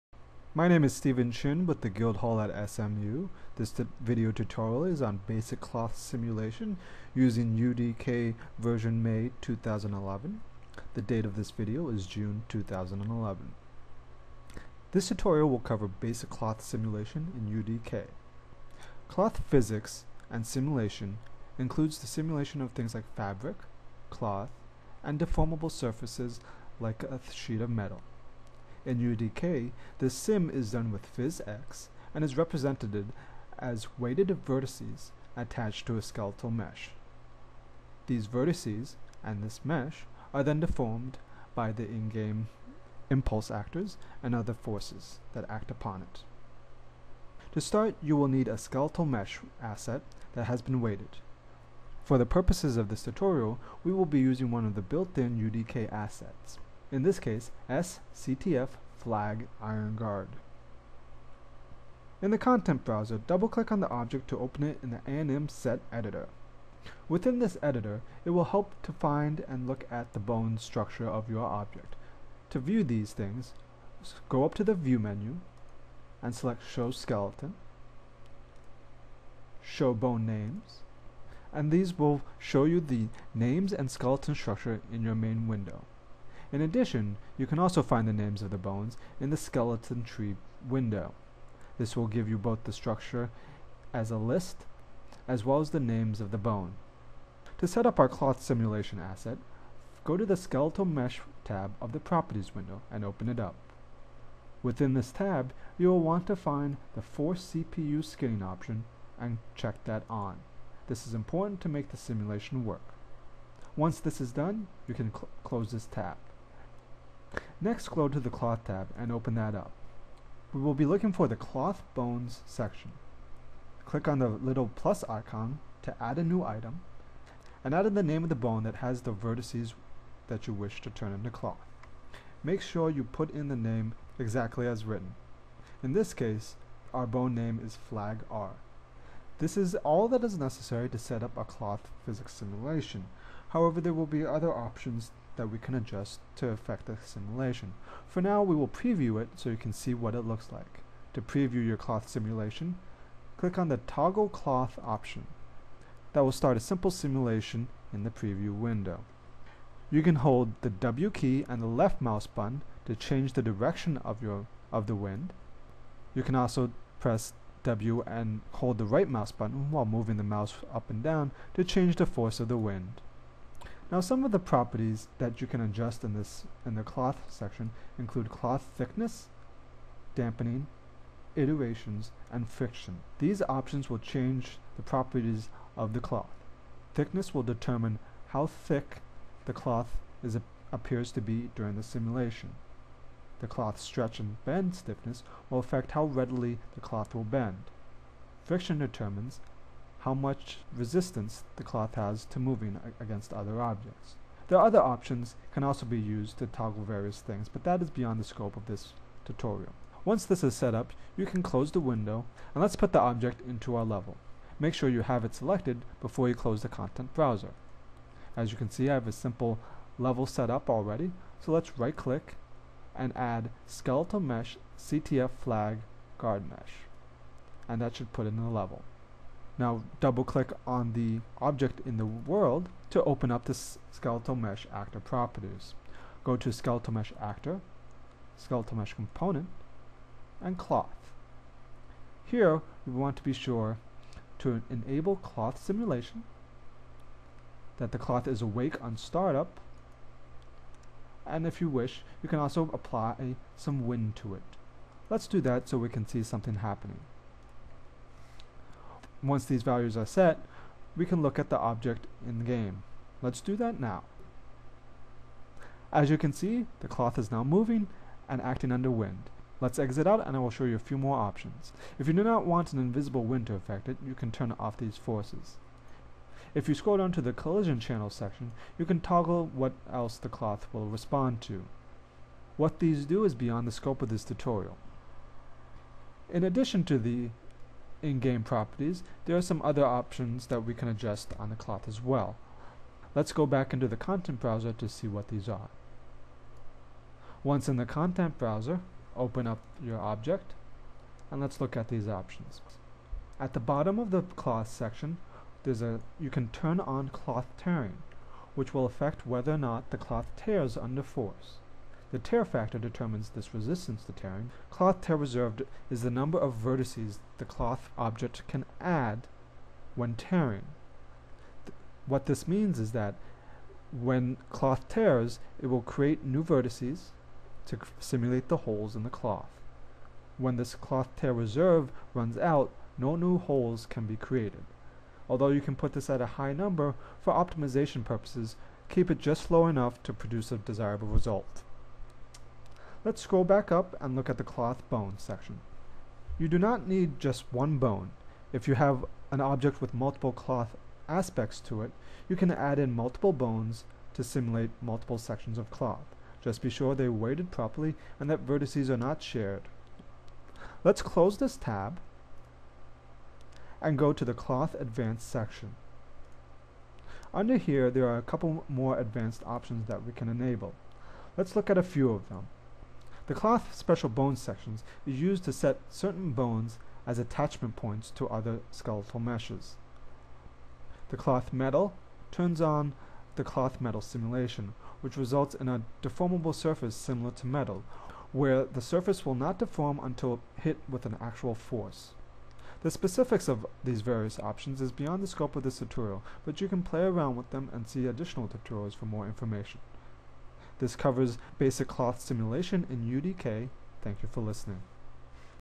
Basic Cloth Simulation Tutorial in UDK
A tutorial for setting up basic cloth simulation in UDK.